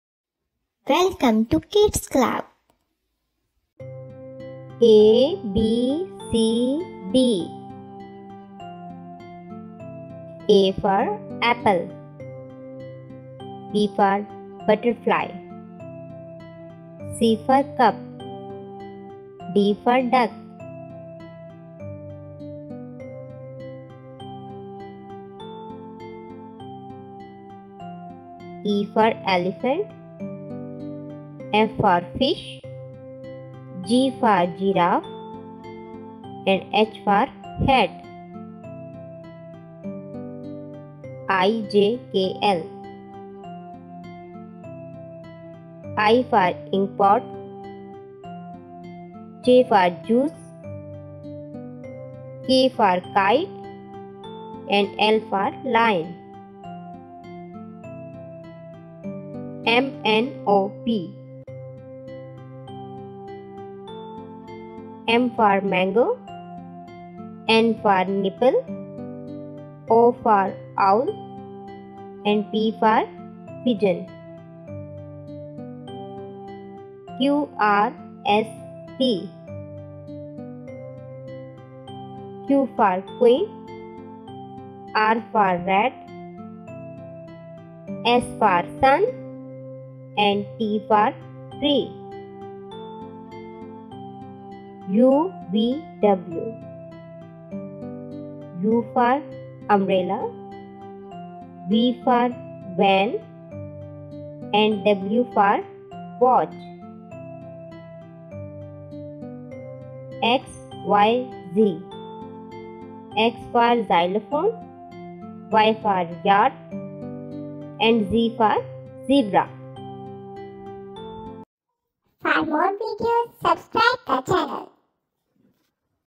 ABCD matching with pictures | Matching alphabets | English Worksheets for Children | Kids Cartoon
You can find corresponding images for abcd letters online or create your own flashcards or posters to help your child learn and remember the alphabet. ABCD matching is fun because you can engage your children for hours.
For example.
A – Apple,
B - Butterfly
C - Cup
D - Duck
E - Elephant
F - Fish
G - Giraffe
H – Hat etc. etc.